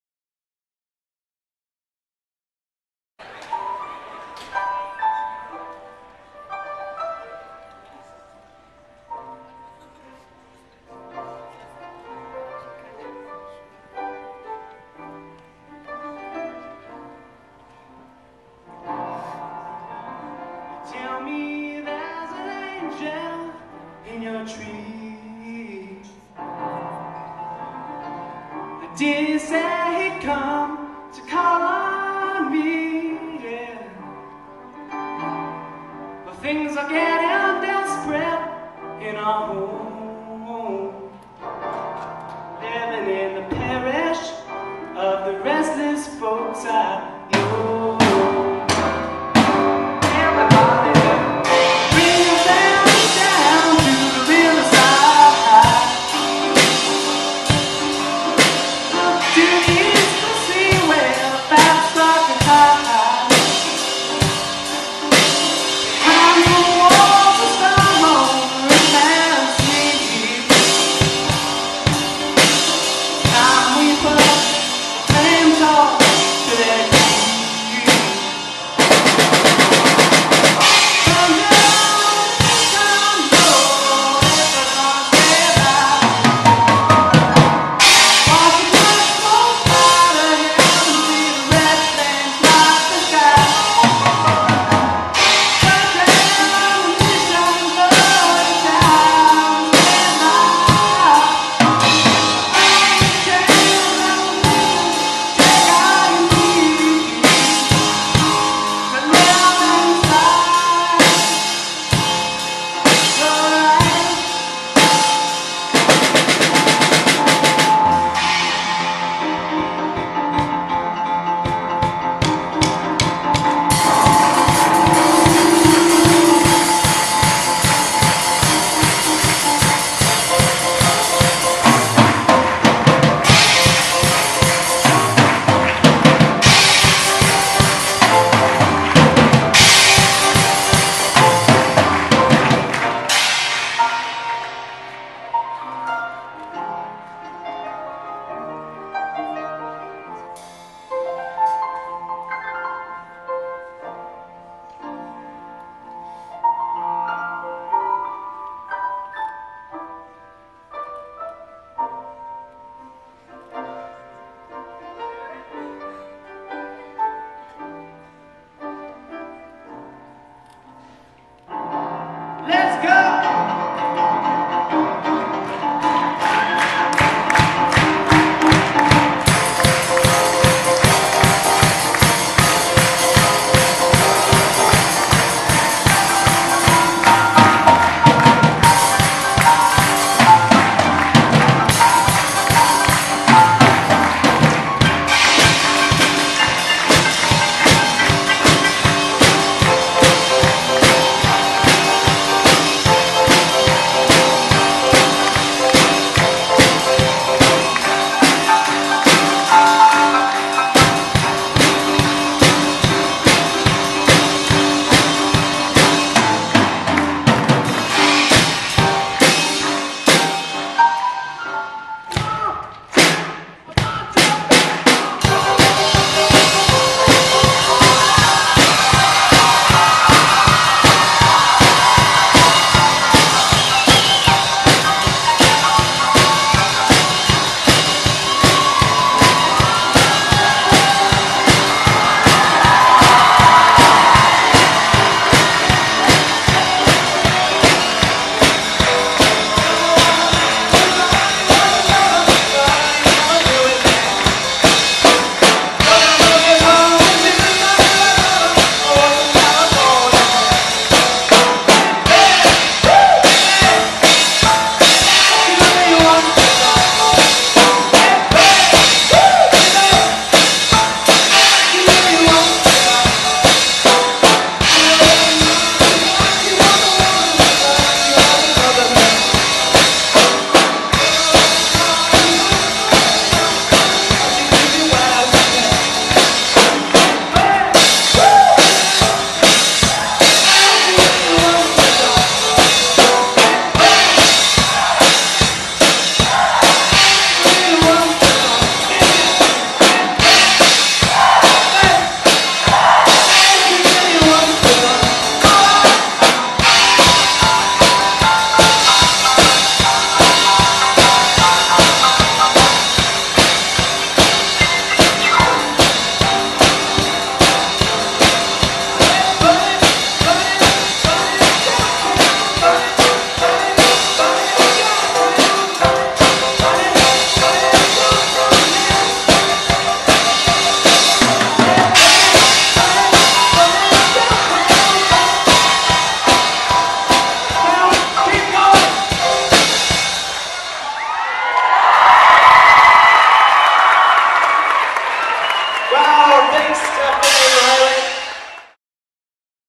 Stefan and Ryland Live at Oly HS 2013
Performing a medley of Elton John and Beatles songs at the Olympia High School assembly on December 20, 2013.